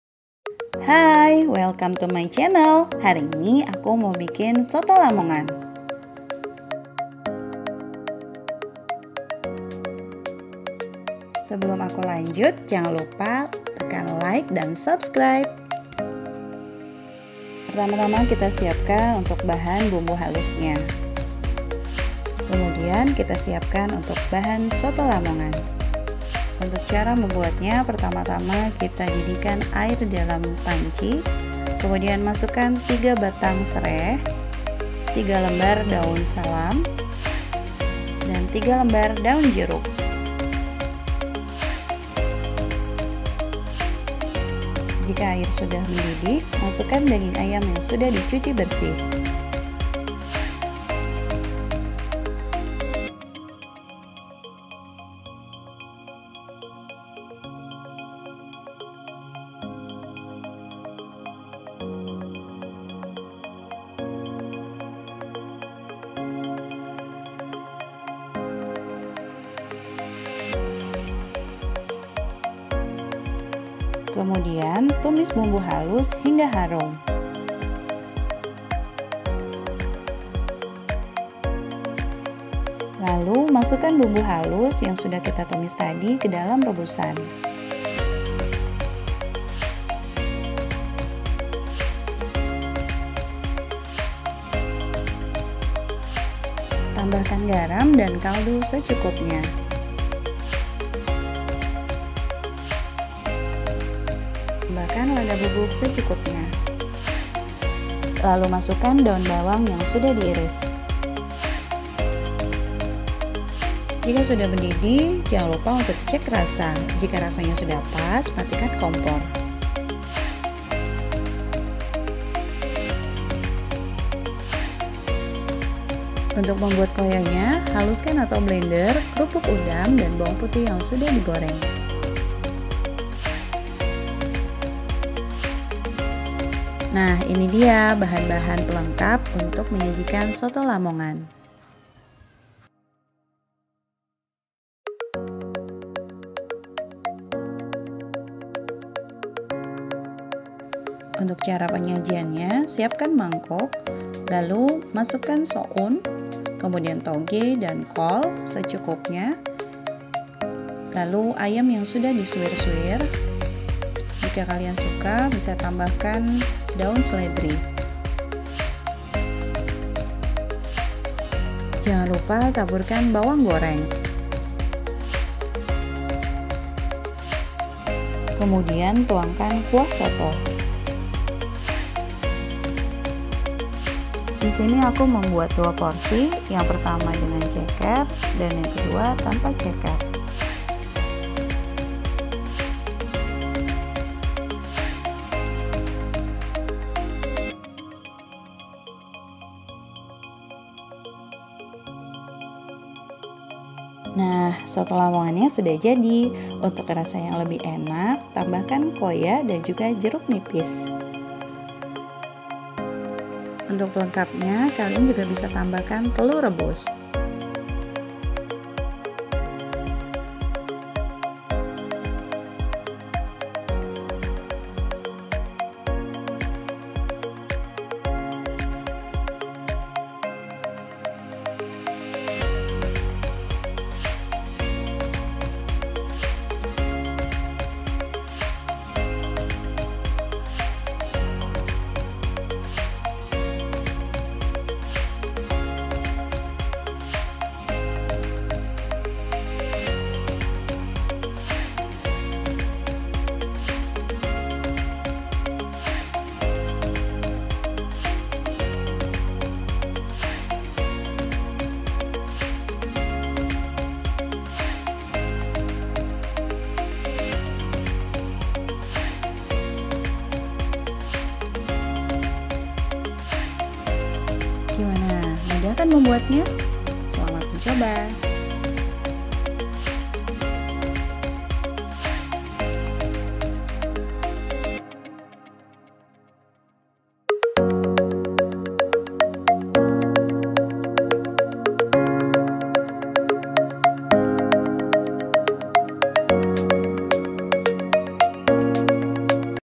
Resep Soto Ayam Lamongan kuah kental
Bahan :
1/2 ekor Ayam
Kol
Tauge
Soun
Daun bawang

Bahan pelengkap:
Jeruk nipis
Bawang goreng
Koya
Daun seledri
Sambal
Kecap

Bumbu Halus:
10 Siung Bawang Merah
8 Siung Bawang Putih
3 btr Kemiri
1 sdm Ketumbar
4 cm Jahe
3 cm Kunyit